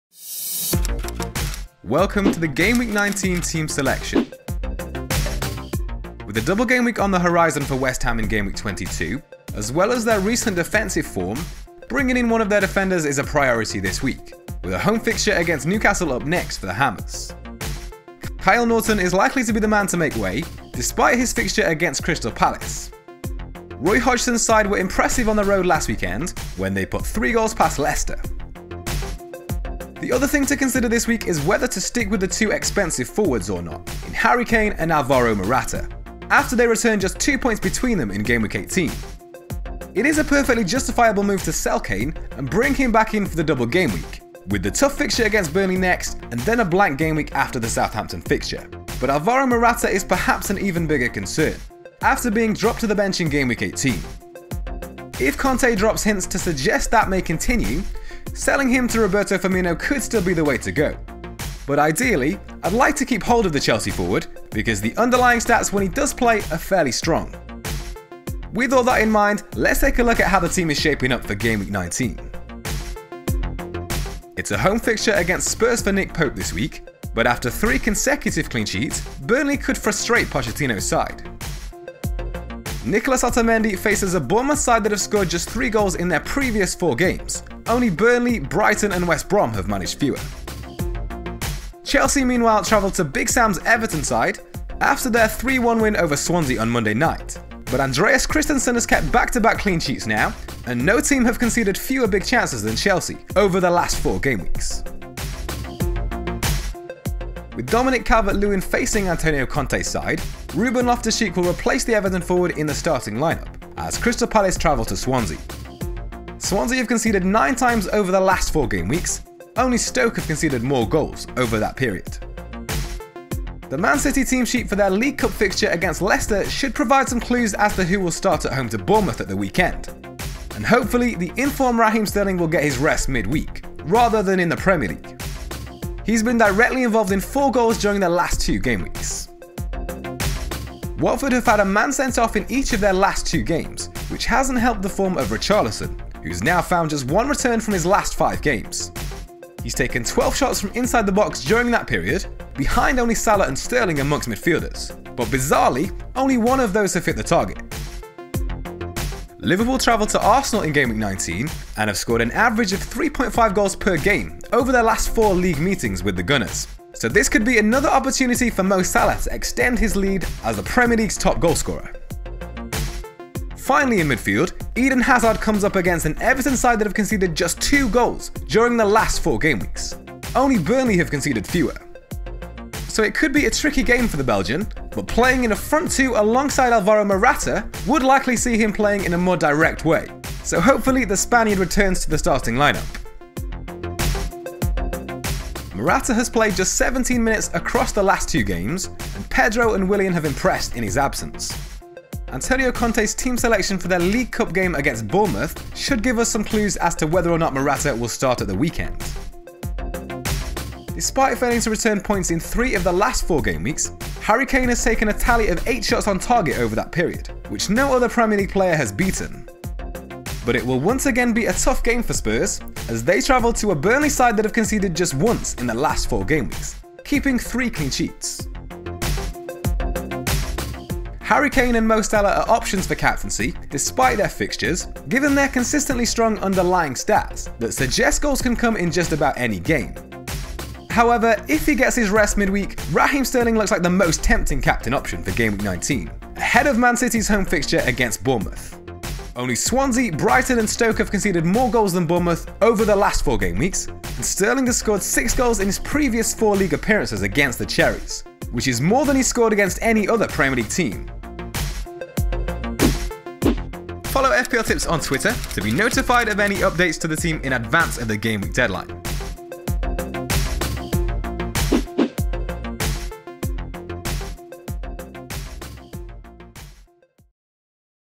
GAMEWEEK 19: TEAM SELECTION | Keep Kane & Morata? | Fantasy Premier League 2017/18
With a double Gameweek on the horizon for West Ham in Gameweek 22, as well as their recent defensive form, bringing in one of their defenders is a priority this week, with a home fixture against Newcastle up next for the Hammers.

Kyle Naughton is likely to be the man to make way, despite his fixture against Crystal Palace. Roy Hodgson’s side were impressive on the road last weekend, when they put 3 goals past Leicester.

The other thing to consider this week is whether to stick with the two expensive forwards upfront, in Harry Kane and Alvaro Morata, after they returned just 2 points between them in Gameweek 18.

It is a perfectly justifiable move to sell Kane and bring him back for the Double Gameweek, with the tough fixture against Burnley next and then a Blank Gameweek after the Southampton fixture, but Alvaro Morata is perhaps an even bigger concern, after being dropped to the bench in Gameweek 18. If Conte drops hints to suggest that may continue, selling him to Roberto Firmino could still be the way to go but ideally, I’d like to keep the Chelsea forward, because the underlying stats when he does play, are fairly strong.

In this video, we'll take a look at how how the FPLtips team will lineup in Gameweek 19. What changes will you make?

► Images and stats are provided by Fantasy Football Fix.

All donations are greatly appreciated and will help ensure the channel can continue to produce content.